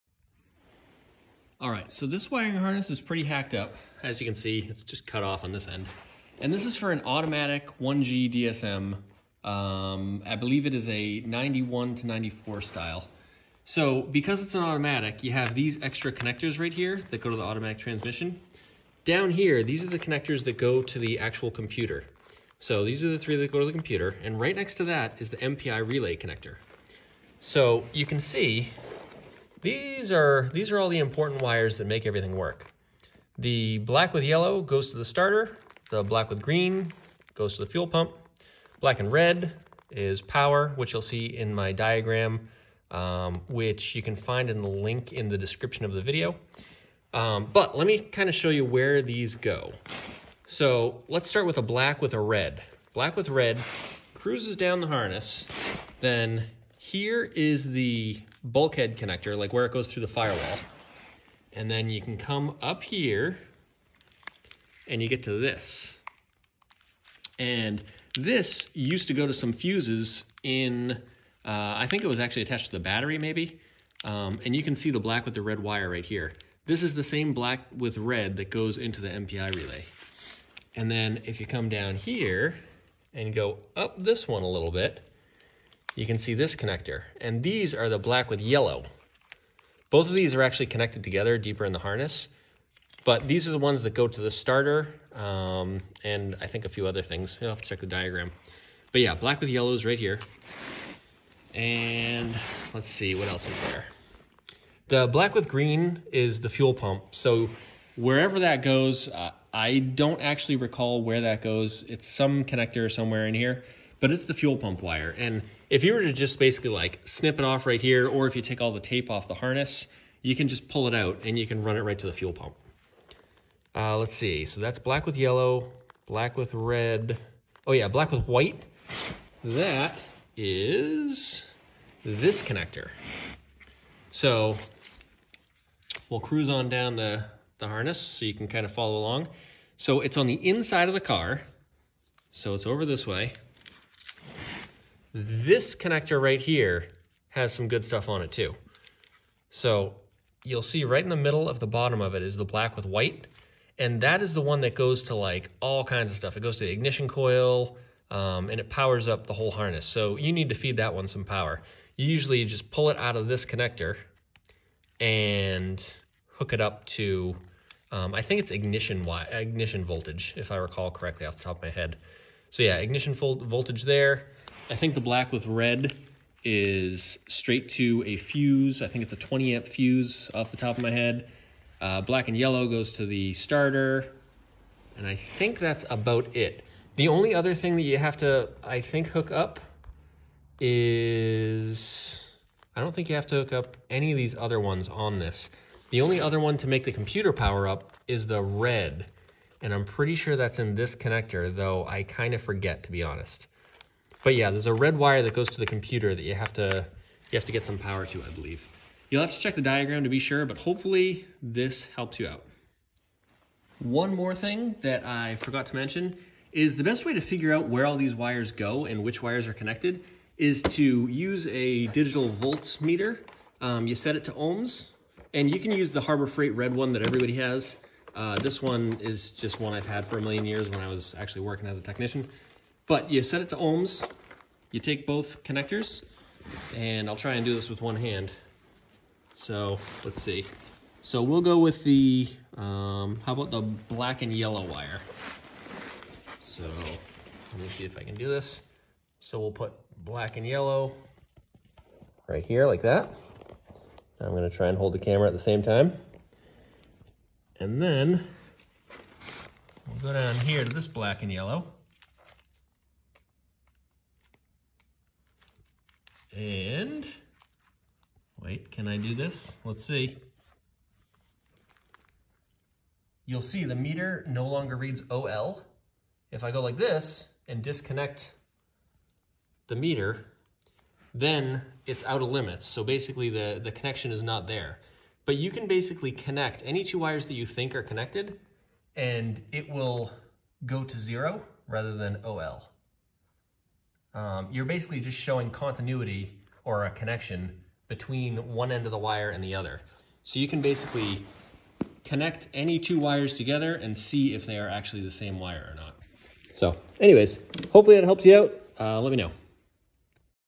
1G 4G63 DSM MPI Relay Fuel Pump ECU Ignition Coil Wiring | Mitsubishi | Eclipse | Talon | Laser
ALL 4G63 DSM MPI WIRING DETAILS IN DESCRIPTION BELOW:
In this video I show you how to wire a 1G DSM 4G63 MPI relay. A 1G DSM includes the 1990, 1991, 1992, 1993, 1994 Mitsubishi Eclipse, Eagle Talon, and Plymouth Laser. When doing a 4G63 turbo engine swap into another car, this MPI relay is usually the most confusing part of the wiring harness.  I do my best to show you what the MPI relay does, and how to properly wire it.

4G63 MPI Wires:
- Pin 1 - Empty.
- Pin 2 - Black w/ green stripe - Goes to the fuel pump from the MPI relay. Many people rewire their fuel pump with 8 gauge wire to make sure it has a strong battery-fed power feed. If doing this, you can use this factory black w/ green stripe on pin 86 of a Bosch relay.  Learn how to properly hard wire your fuel pump in this video
- Pin 3 - Black w/ white stripe - Should be given Ignition power. This wire on the MPI relay also goes to pin 110 at the ECU, the Ignition Coil and Power Transistor.
- Pin 4 - Red wire - Goes to Pin 107 and 102 on the ECU. It also jumps over to Pin 5 on the MPI relay, and sends power to the Mass Airflow Sensor, CAS, Wastegate Solenoid, ISC, O2 Sensor, Injector Resistor, Purge Control Solenoid, Fuel Pressure Solenoid, and EGR Solenoid.
- Pin 5 - Connects to Pin 4 on the MPI relay
- Pin 6 - Black wire - Goes to ground.
- Pin 7 - White w/ red stripe - goes to pin 56 at the ECU
- Pin 8 - Black w/ blue stripe - Goes to Pin 63 and 66 at the ECU.
- Pin 9 - Black w/ yellow stripe - This Goes to Pin 108 at the ECU AND the small wire on the engine's starter relay. This should ONLY be given power when the engine is cranking from the starter. The moment you let go of the key (when the engine starts up), this wire should go dead.
- Pin 10 - Black w/ red stripe - This should go to a 20A fuse fed right off the battery.